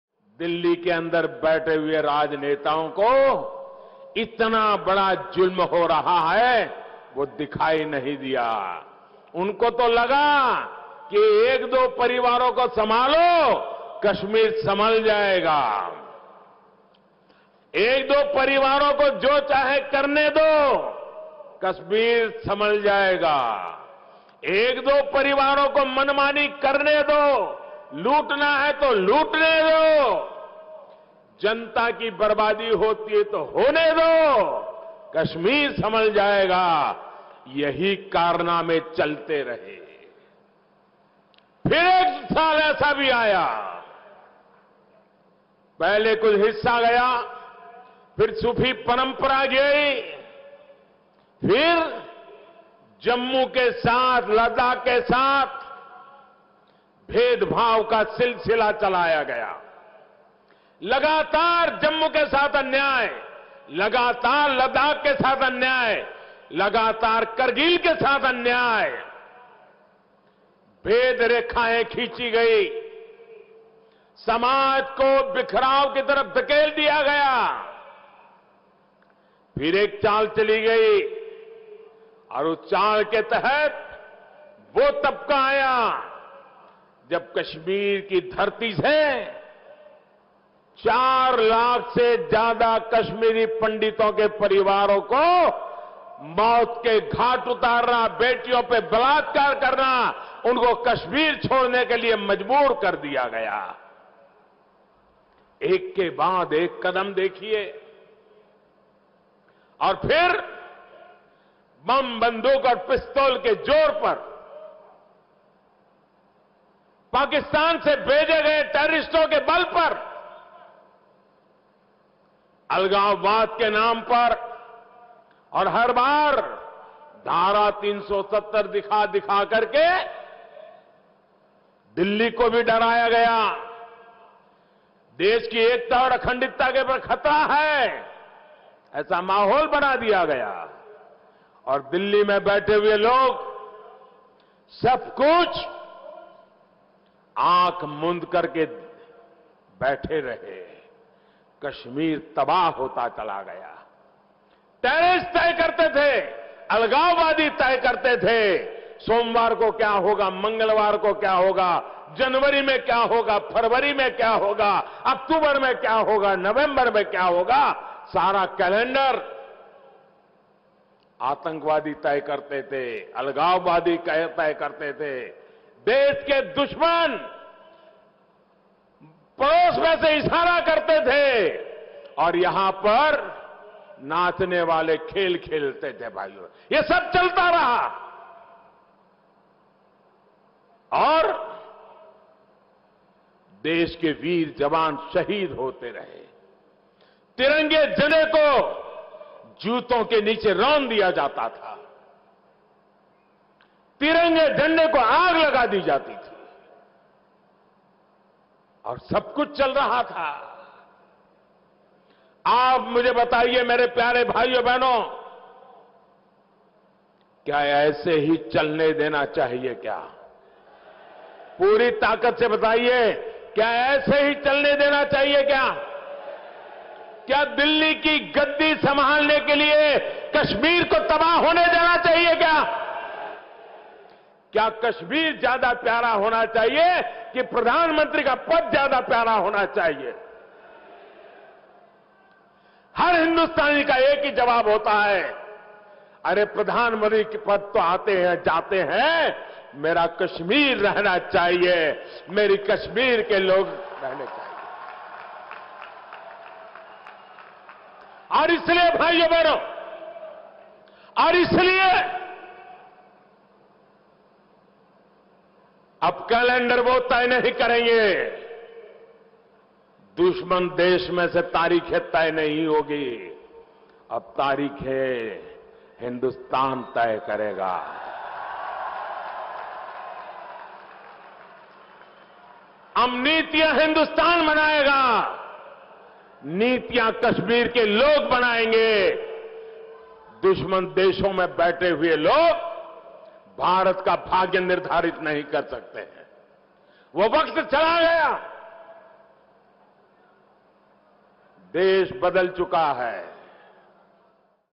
अब कैलेंडर वो तय नहीं करेंगे। अब तारीखें हिंदुस्तान तय करेगा: पीएम मोदी, हरियाणा
पाकिस्तान से भेजे आतंकवादियों के बल पर, अलगाववाद के नाम पर और अनुच्छेद 370 दिखाकर डराया जाता रहा।

पहले सारा कैलेंडर आतंकी तय करते थे।

अब कैलेंडर वो तय नहीं करेंगे। अब तारीखें हिंदुस्तान तय करेगा: पीएम मोदी, ऐलनाबाद, हरियाणा, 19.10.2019